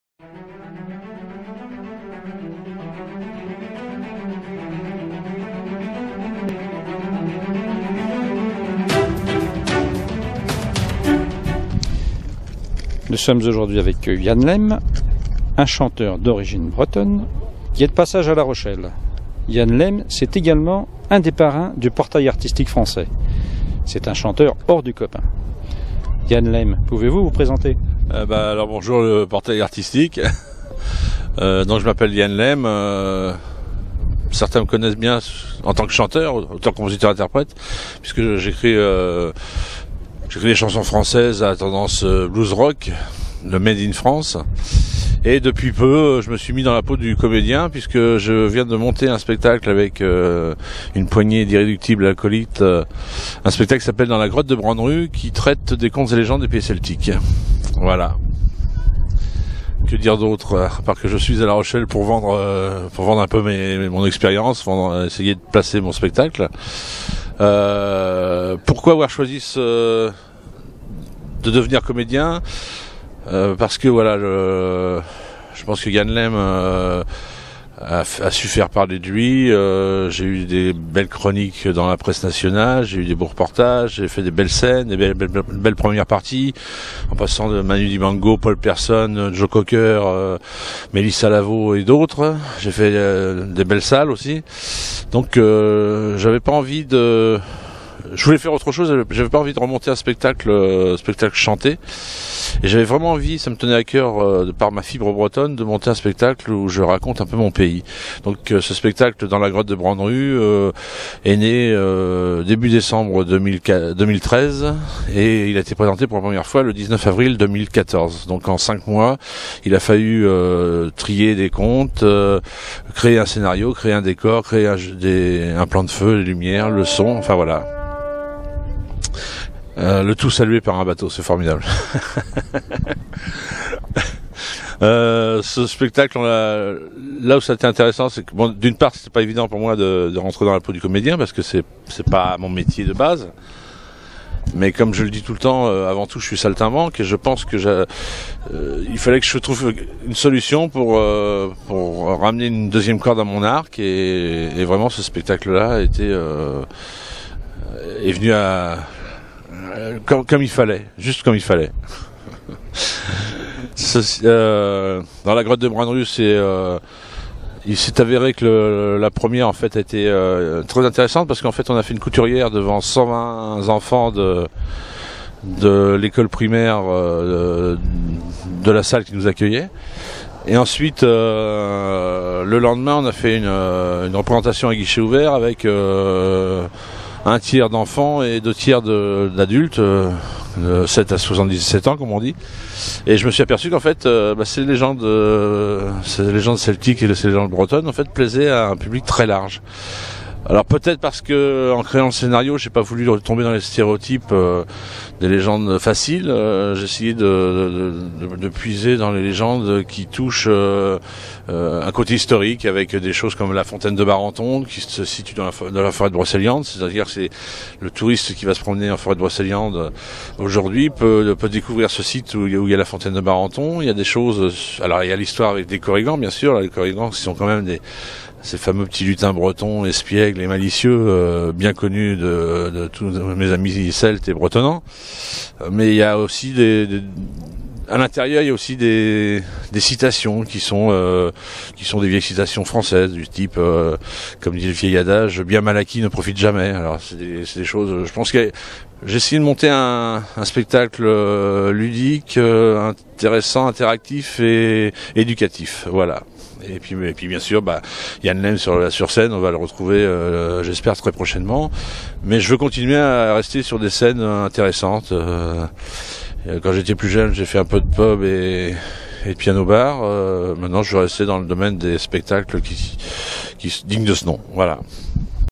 Yann Lem
De passage à La Rochelle, Yann Lem, chanteur breton nous parle d'un spectacle dont il est le principal interprète, un artiste multi carte !